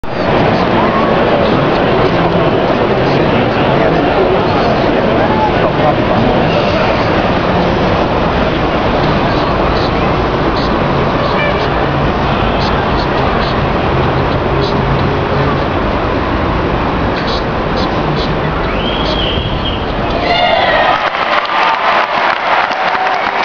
Jeremy Staunton Penalty Kick - Leicester Tigers Vs Saracens 08/05/10
Jeremy Staunton kicks a penalty at Welford Road Stadium 08/05/10